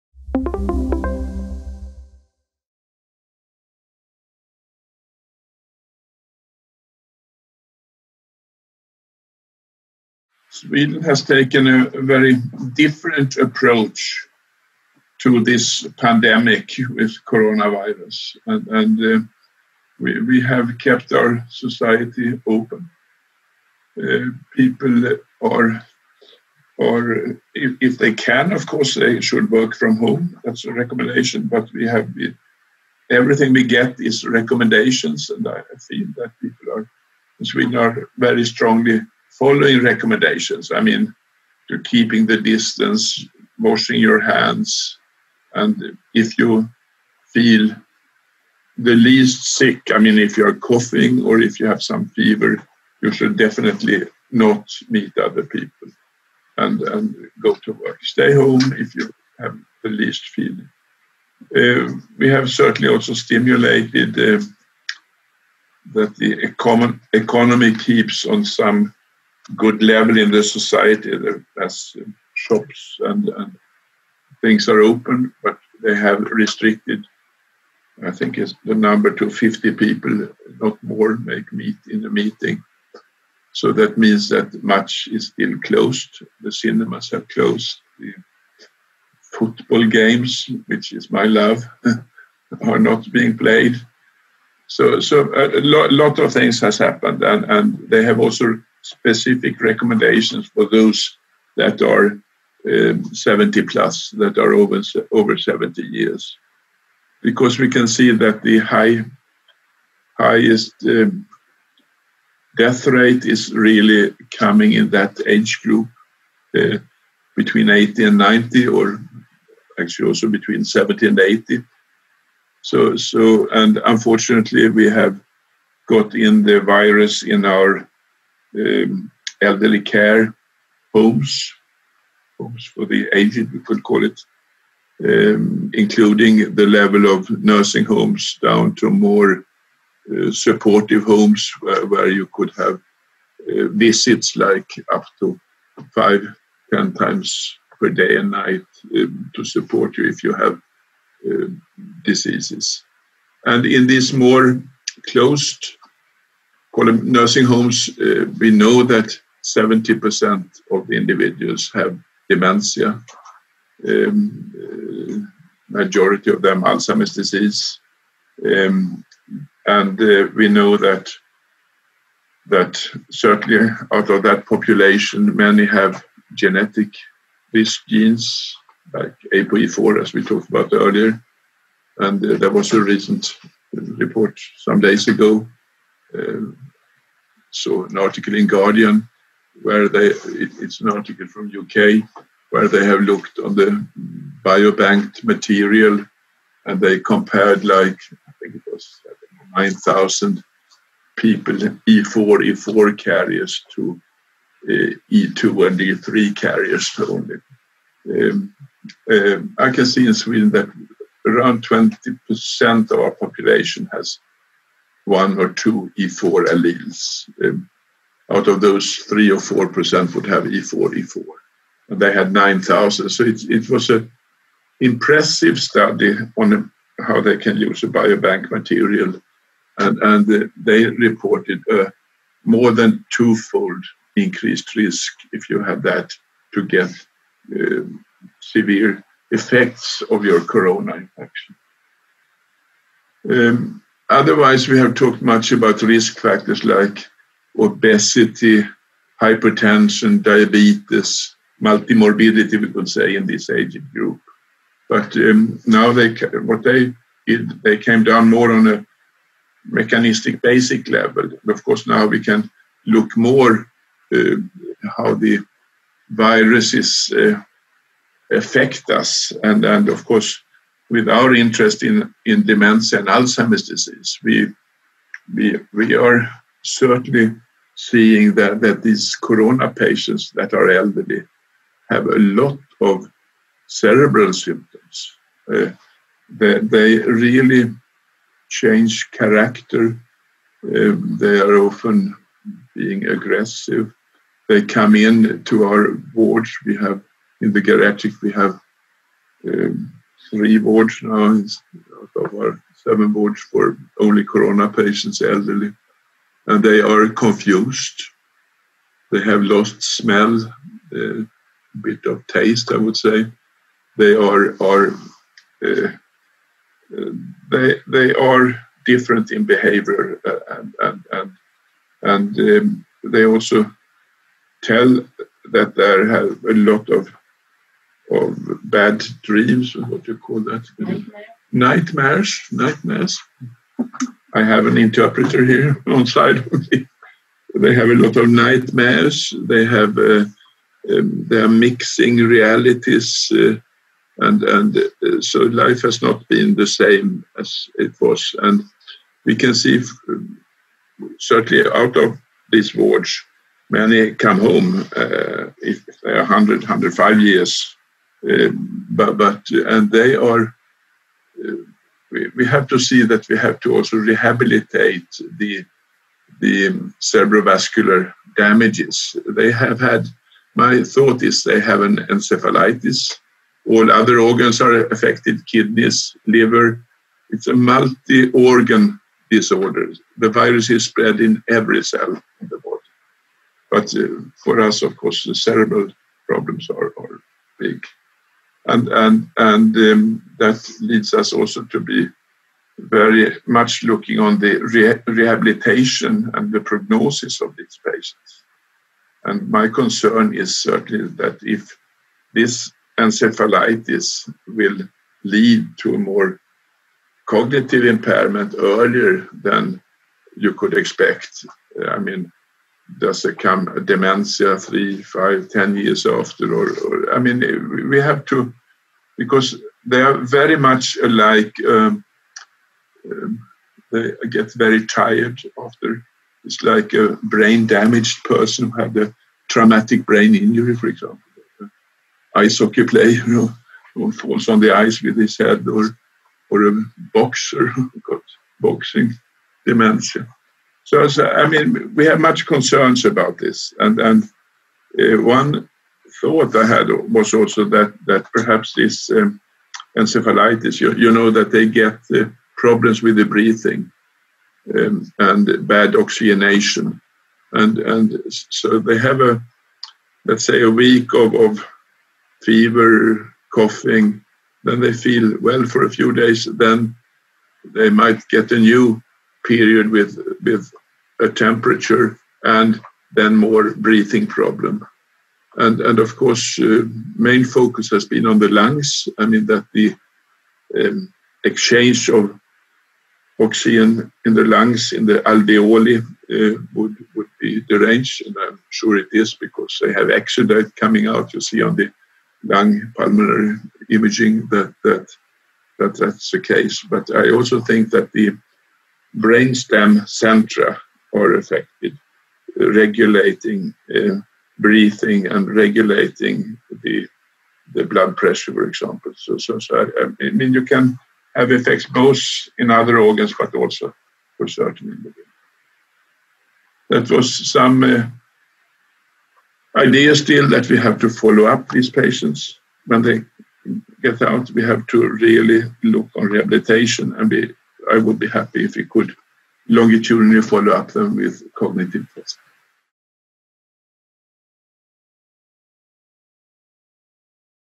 Bengt Winblad, EAN 2020 – Swedish Geriatric Neurologist Perspective on COVID-19
Always a pleasure to catch-up with Editorial Board member, Bengt Winblad (Karolinska University Hospital and Karolinska Institutet, Stockholm, Sweden), who chatted with us on several topics including his perspective on the COVID-19 pandemic as a Swedish geriatric neurologist.

Question
What are your thoughts on the COVID-19 pandemic as a Swedish geriatric neurologist? (0:05)

Speaker disclosure: Bengt Winblad has nothing to disclose in relation to this video interview.

Support: Interview and filming supported by Touch Medical Media.

Filmed at the 6th Congress of the European Academy of Neurology (EAN) and 1st EAN Virtual Congress, May 2020.